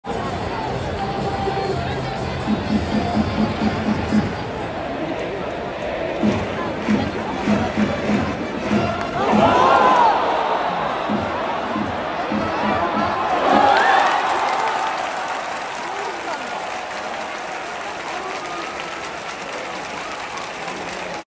オリックス 杉本裕太郎 超ファインプレーを見せる！！
2024年6月8日　日本生命セ・パ交流戦　オリックスバファローズ対読売ジャイアンツ　2回戦